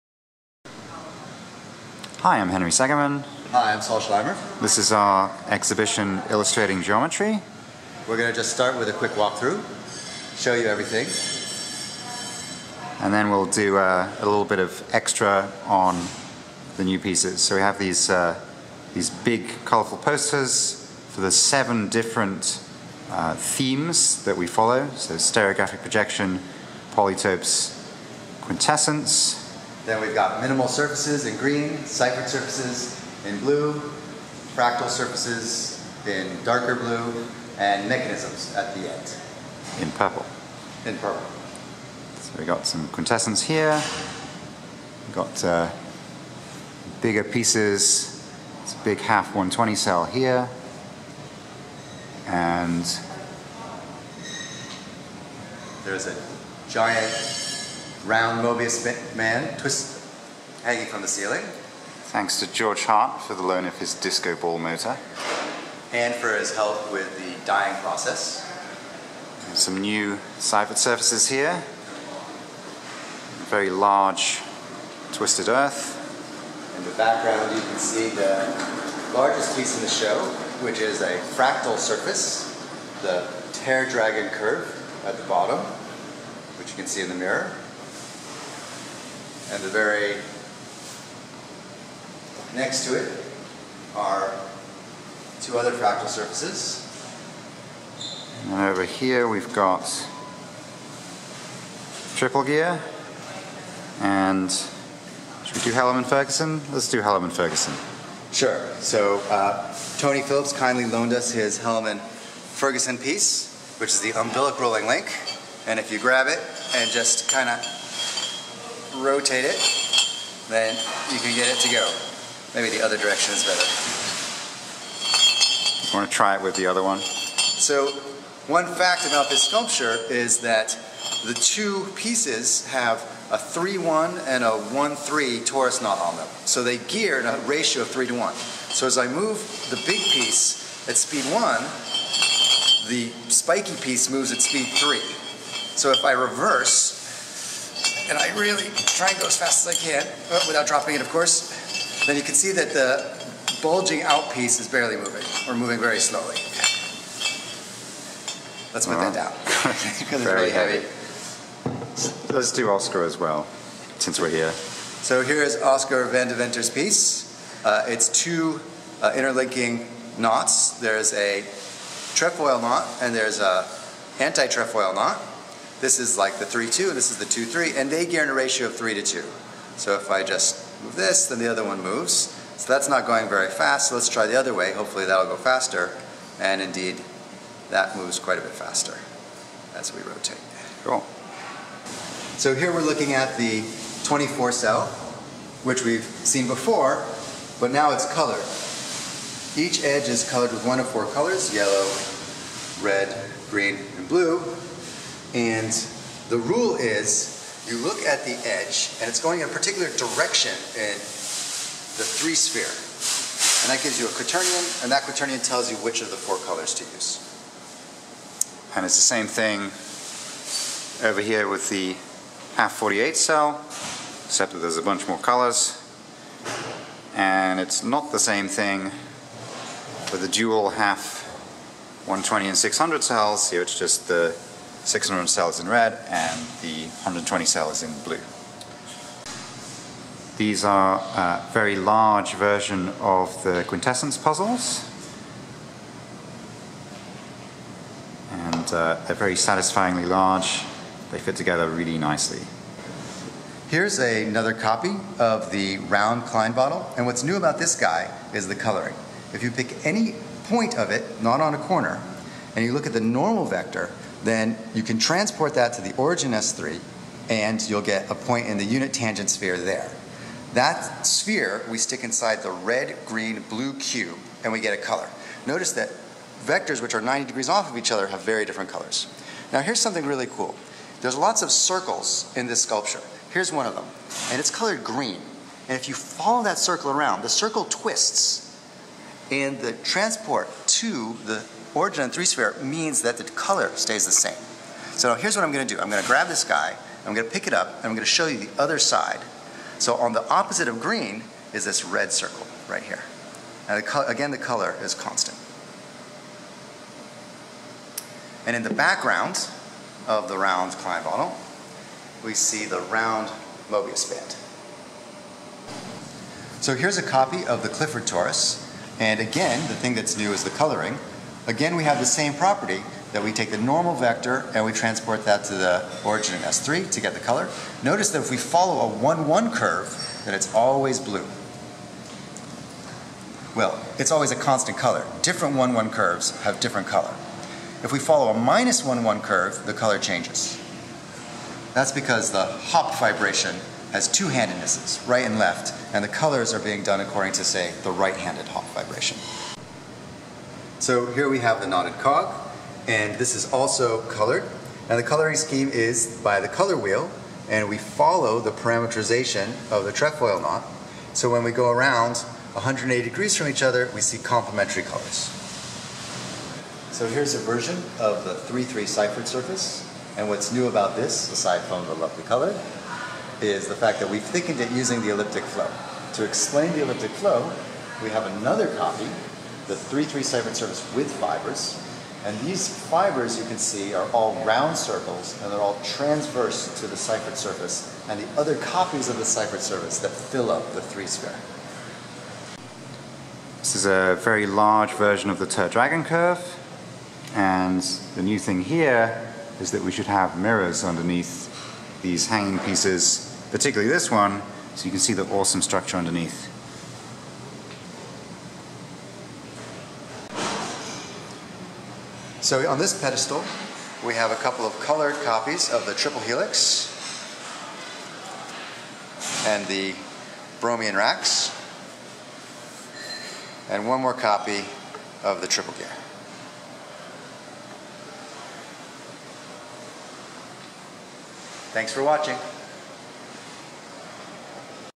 "Illustrating Geometry" exhibition walkthrough
This is Saul Schleimer's and my art exhibition at the Simons Center for Geometry and Physics at Stony Brook.

Many thanks to Maria Froehlich, Tim Young, Katherine Schwarting and Margaret Schedel for all of their help, and to George Hart for helping us with dyeing some of the sculptures. Thanks also to the Museum of Mathematics and Tony Phillips for the loan of their artworks for the show.